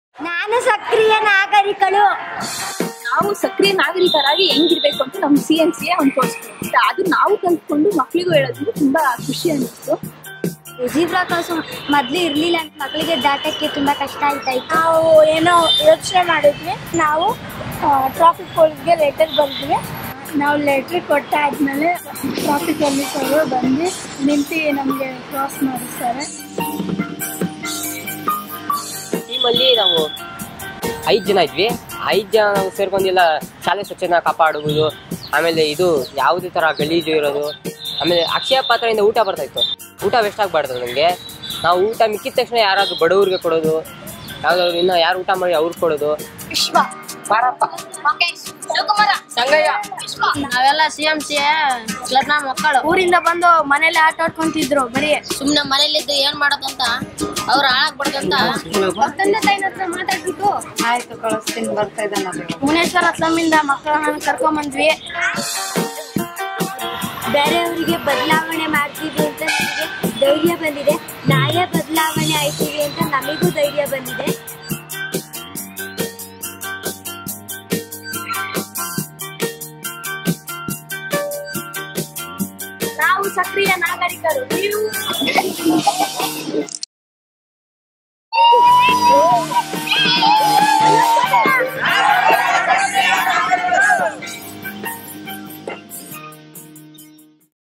CMCA's Active Citizens
CMCA engages with young people in government and private schools across India. Children are assisted to develop skills and knowledge to grow into concerned, thinking and active citizens.
These thoughtful youngsters from our government school  program  took action to bring about change in their school and their community.